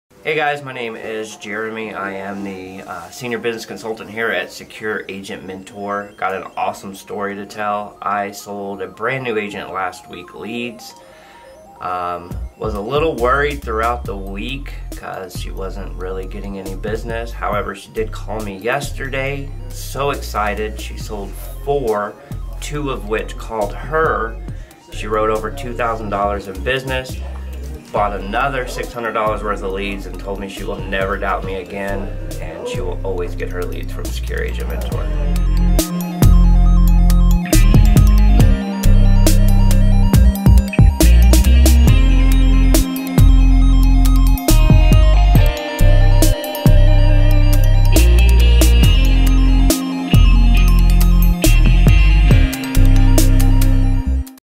Brand NEW Insurance Agent Writes Over $2,000 in 1 Week!
The amount of success stories we've heard from agents in the past week alone is INSANE! 💰💰

The new features we've implemented in our lead process is making our leads better than ever, making it easier for you to set appointments and write policies! 📝👍

Give one of our business consultants a call today at 833-40-AGENT to see how we can help you! ☎️